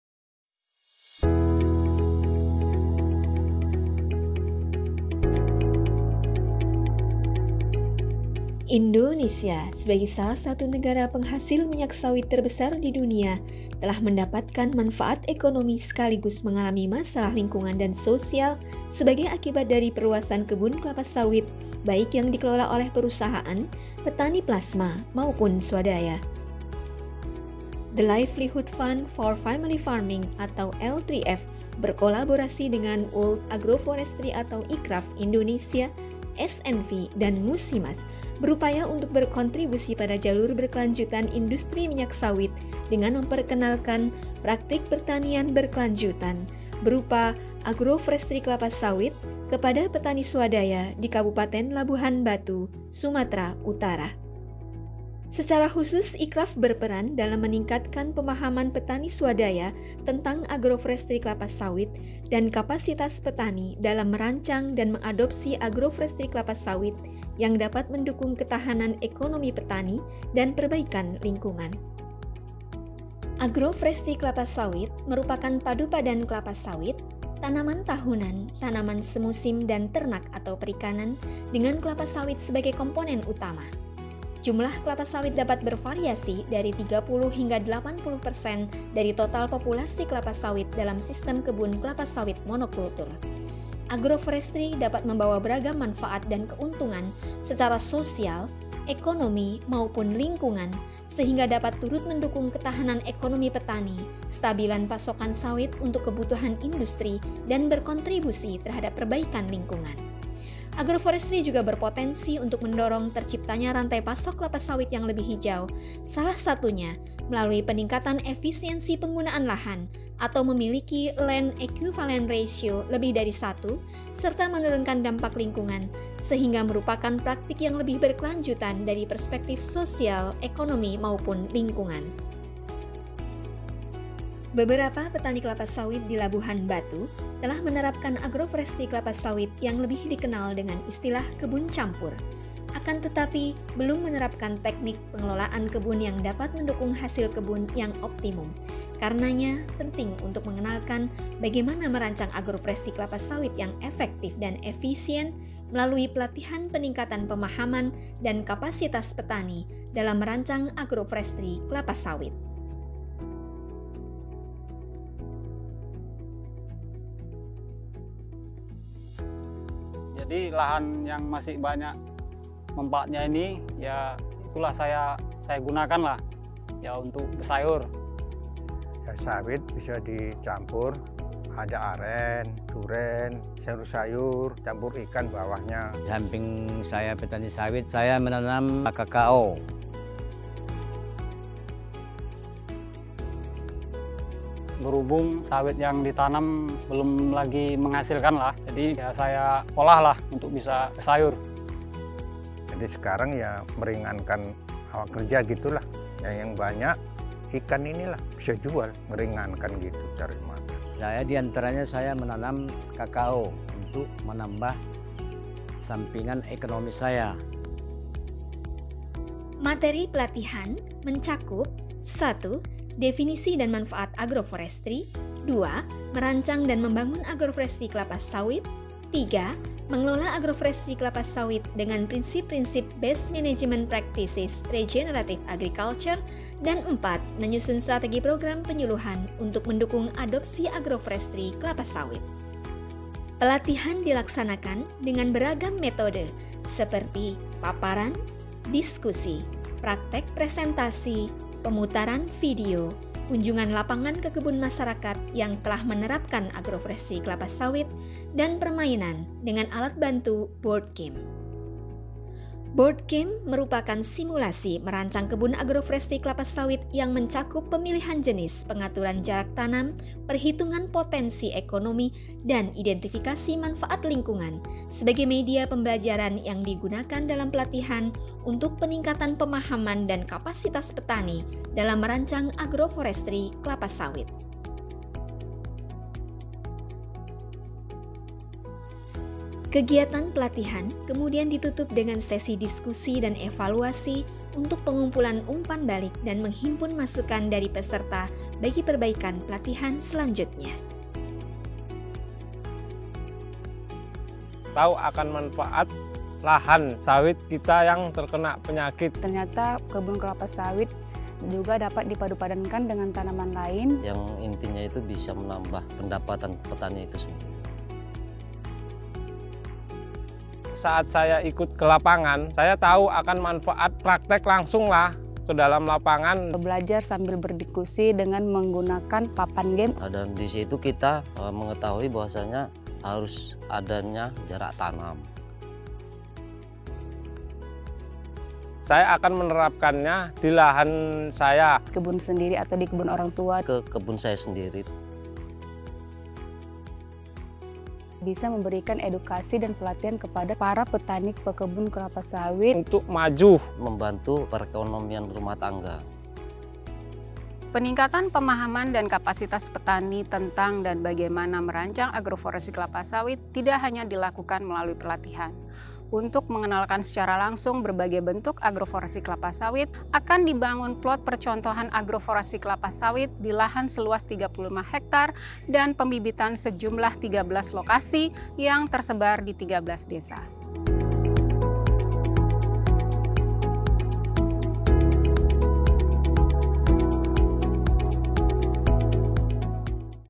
Peningkatan Kapasitas Petani Kelapa Sawit Kabupaten Labuhanbatu, Sumatera Utara
Video ini menyajikan informasi tentang kegiatan AgroforestriSawit yang dilaksanakan oleh ICRAF Indonesia bersama para mitra SNV dan MUSIM MAS, dengan dukungan The Livelihoods Fund for Family Farming (L3F), kepada petani swadaya kelapa sawit di Kabupaten Labuhanbatu, Sumatera Utara. Kegiatan ini merupakan bagian dari proyek Biodiverse and Inclusive Palm Oil Supply Chain (BIPOSC).

Video juga memaparkan tentang kegiatan peningkatan pemahaman dan kapasitas petani swadaya kelapa sawit yang dilakukan melalui pelatihan, serta harapan peserta pelatihan terkait pengembangan AgroforestriSawit di Labuhanbatu.

This video presents information about the oilpalmagroforestry activities carried out by ICRAF Indonesia with partners SNV and MUSIM MAS, with the support of The Livelihoods Fund for Family Farming (L3F) for independent oil palm smallholders in Labuhanbatu District, North Sumatra. This activity is part of the Biodiverse and Inclusive Palm Oil Supply Chain (BIPOSC) project.

The video also describes the activities in increasing the understanding and capacity of independent oil palm smallholders carried out through training, as well as the expectations of the training participants for the development of oilpalmagroforestry in Labuhanbatu.